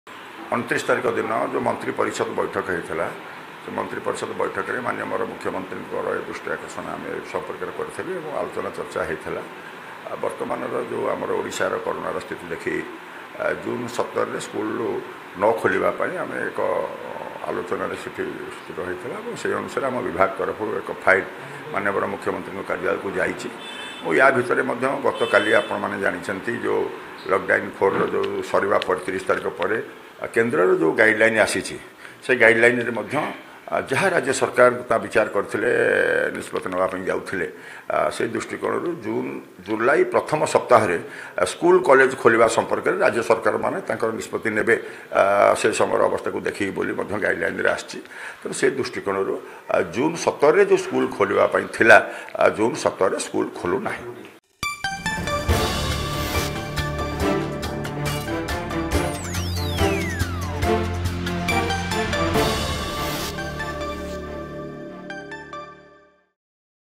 ରାଜ୍ୟରେ ଖୋଲିବନି ସ୍କୁଲ୍ ....
କୋରୋନା ପାଇଁ ରାଜ୍ୟରେ ଆସନ୍ତା ଜୁନ୍ 17 ତାରିଖରୁ ଖୋଲିବନି ସ୍କୁଲ୍ । ଏ ନେଇ ସ୍କୁଲ ଓ ଗଣଶିକ୍ଷା ମନ୍ତ୍ରୀ ସମୀର ଦାଶଙ୍କ ପକ୍ଷରୁ ସୂଚନା...

Category
News and Politics
Licence